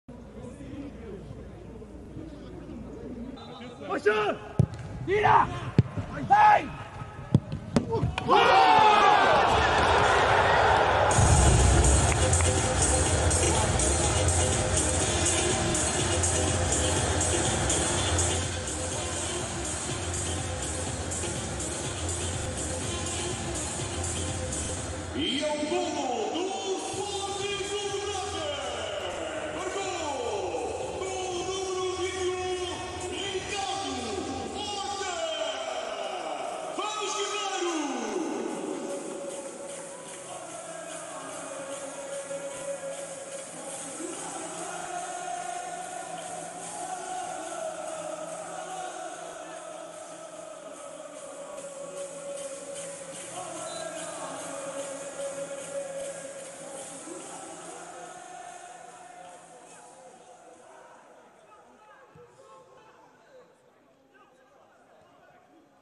Goal | Golo Ricardo Horta: SC Braga (1)-1 FC Porto (Liga 19/20 #34)
Livre lateral de Trincão, Danilo corta para a entrada da área onde aparece Ricardo Horta a disparar com a bola a desviar ainda num jogador portista e a trair Diogo Costa.